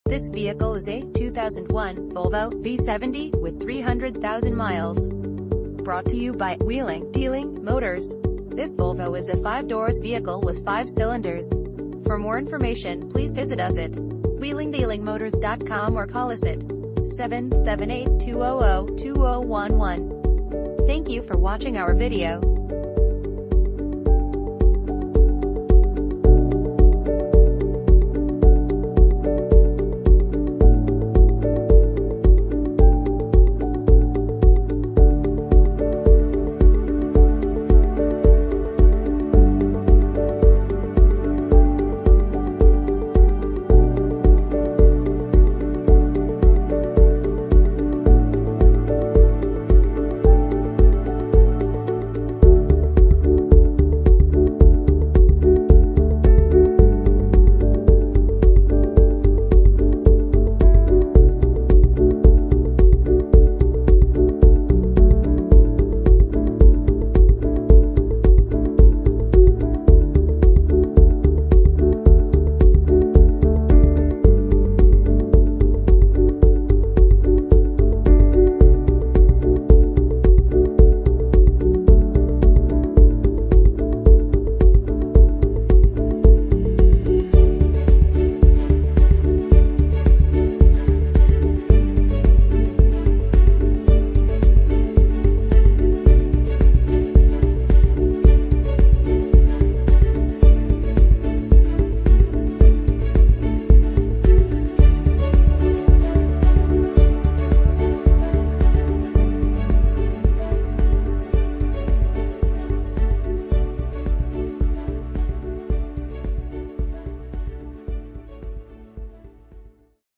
2001 Volvo V70 - Wheeling Dealing Motors
Wheeling Dealing Motors
2540 Bowen Road
Nanaimo, BC V9T3L3

2001 Volvo  V70

2 owners, Clean title, Extended Warranty Available, 300,000 kilometers, Automatic, Ash Gold Metallic exterior, Wagon, FWD: Front Wheel Drive, 5 doors, 5 cylinders, Gasoline.